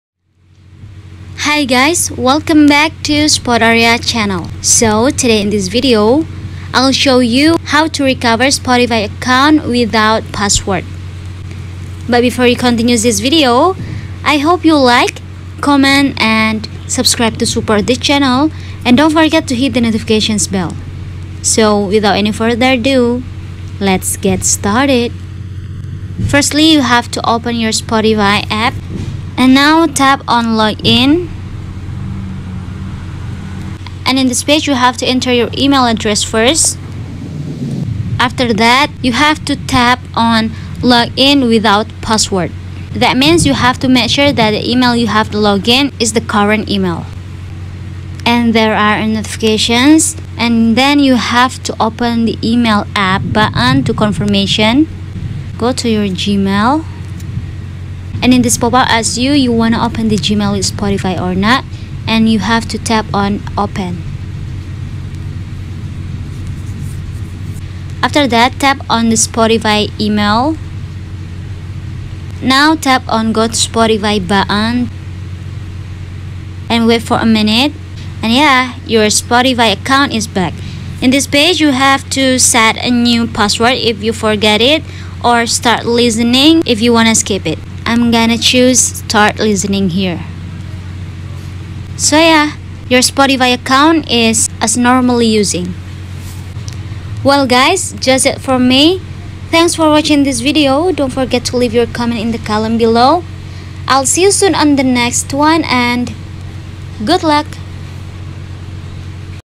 How To Recover Spotify Account Without Password
Hi guys, in this video I'll show you how to recover spotify account without password.

*this video is related to :
how to recover spotify account forgot password
how to recover spotify account